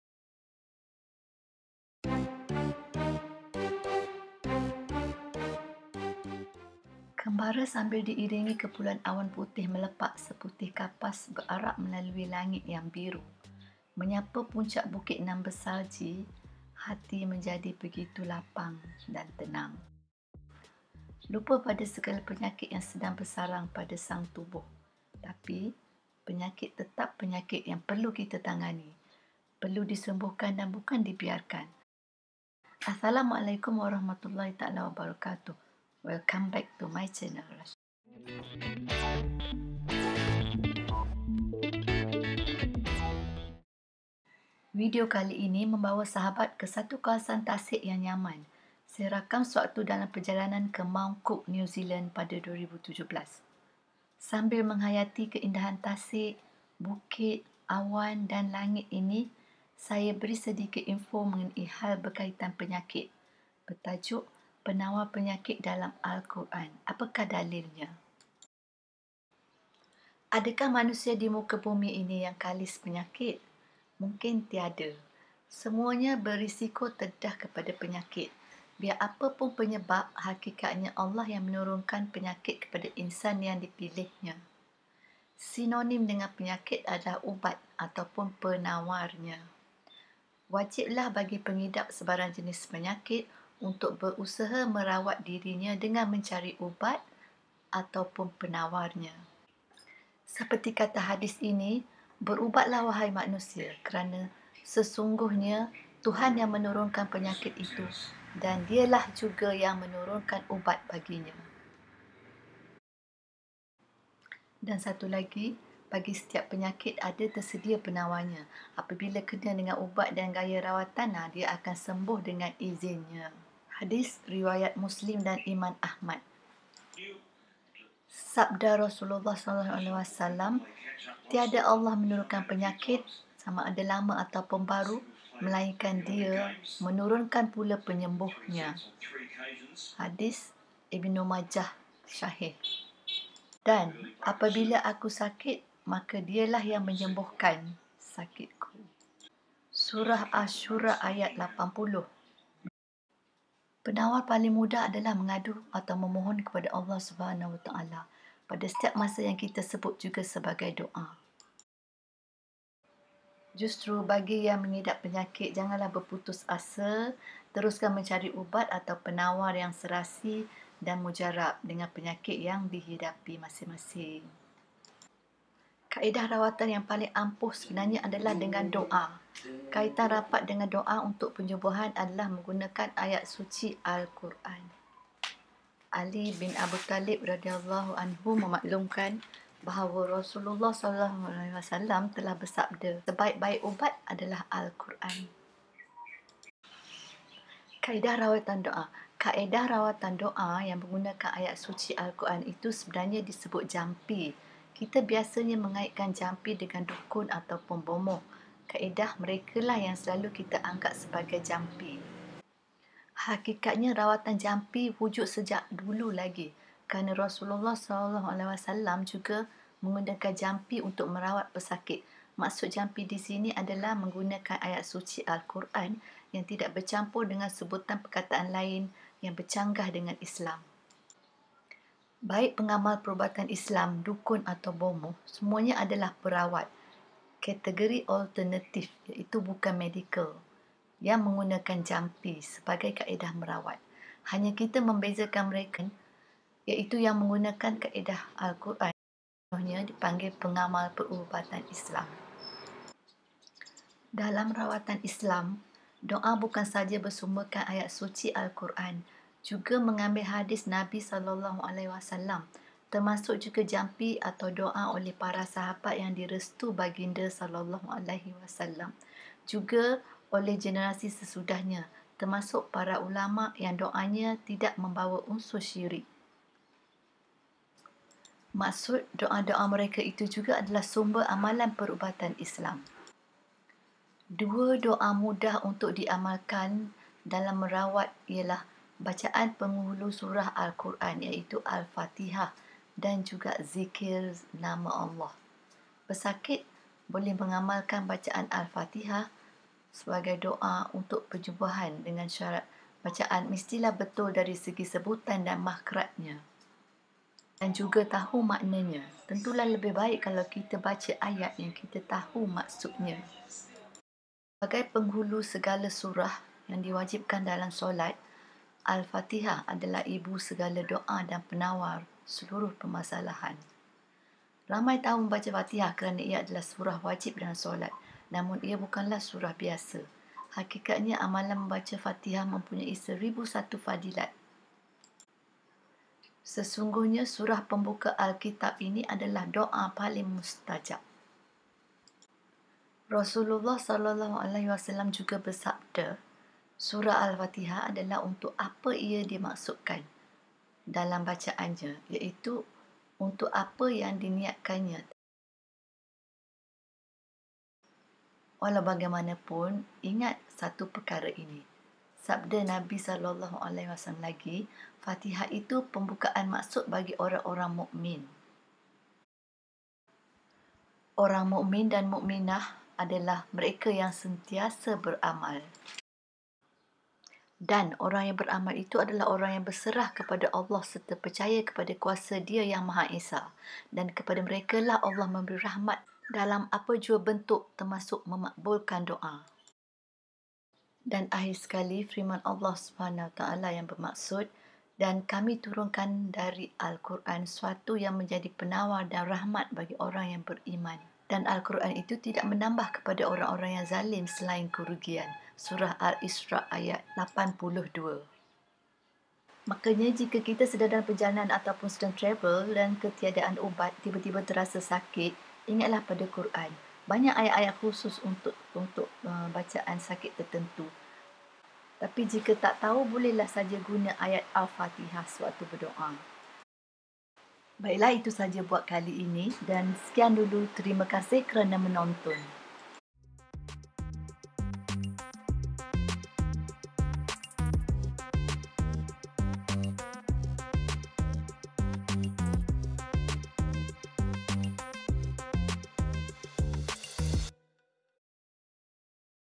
Penawar Penyakit Dalam al Quran Ini Dalilnya | Rawatan Dalam Islam | Latar New Zealand
Penawar Penyakit Dalam al Quran Ini Dalilnya | Rawatan Dalam Islam  mov | Latar New Zealand

Assalamualaikum sahabat maya semua, kali ini nak berkongsi info mengenai dalil yang membuktikan al Quran itu adalah penawar kepada penyakit. Allah memberikan penyakit dan Dia juga memberi penawarnya.